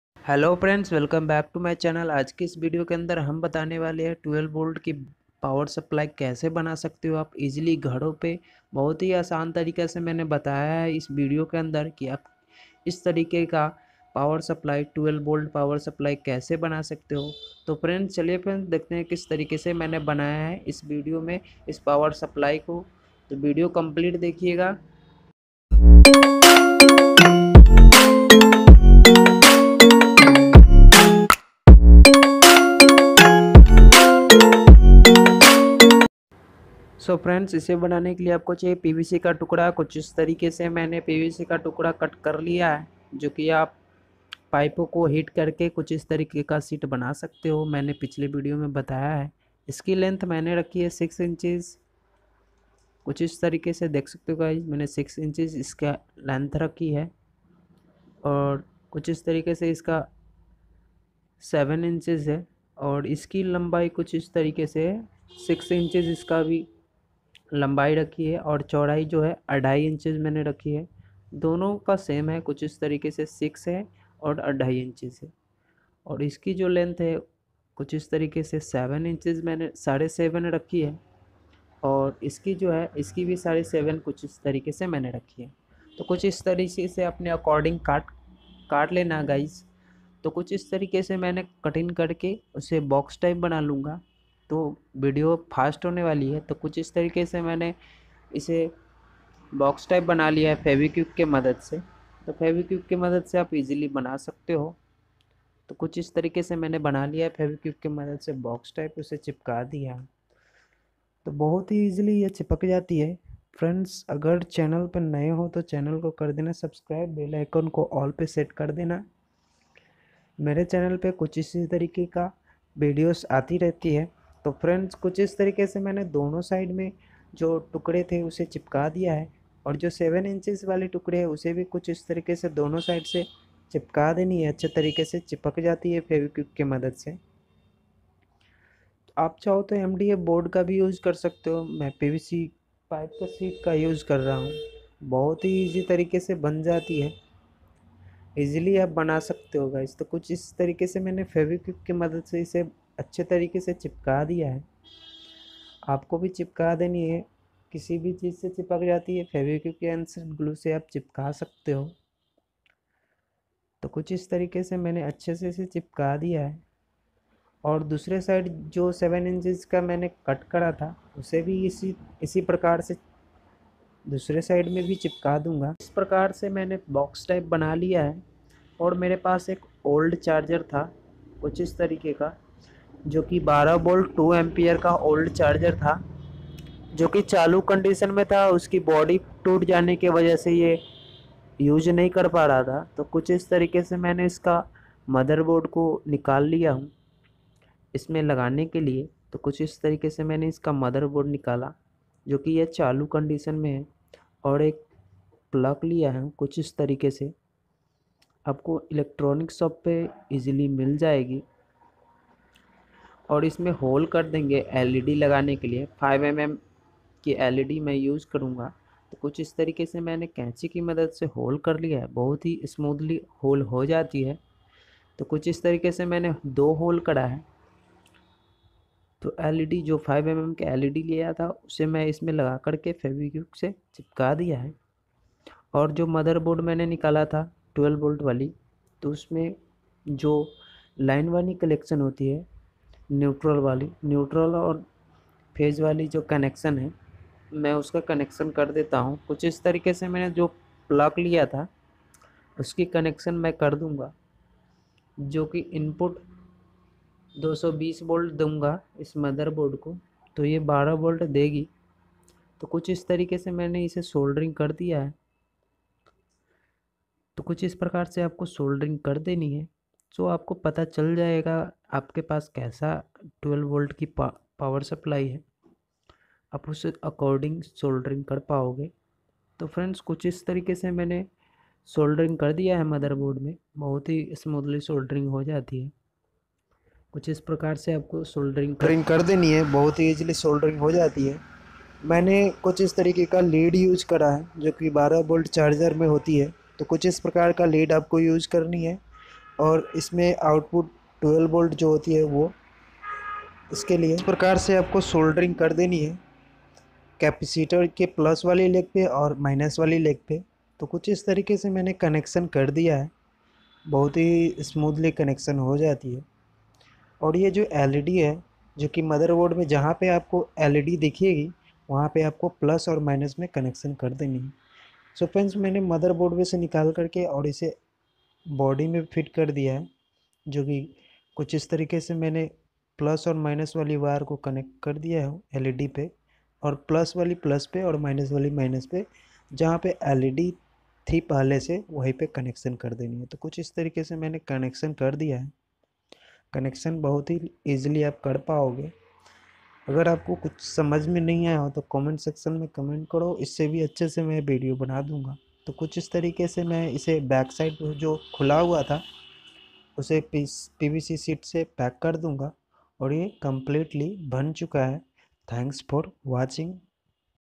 12v battery charger kaise banaen"how to make powerful 12 volt battery charger at home
hello friends welcome to my channel this video very important and useful gadget making video how to make battery charger 12 volt power supply at home M2 friends as kis video can the hum batane wale ki aap kaise 12 volt car battery bana sakte ho battery charger bana sakte ho